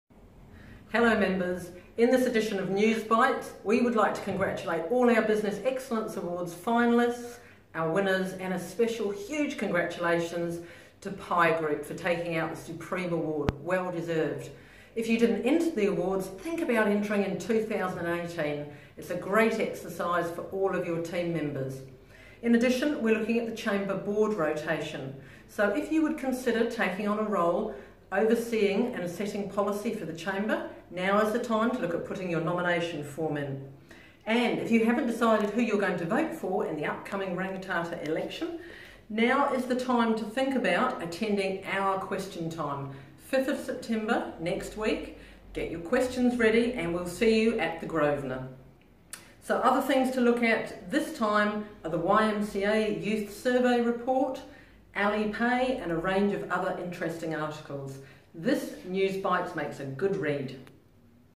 NewsBites Overview August 30 3017
The Chief Executive of the South Canterbury Chamber of Commerce gives an overview of this edition of NewsBites.  NewsBites is a fortnightly newsletter to members of the South Canterbury Chamber of Commerce.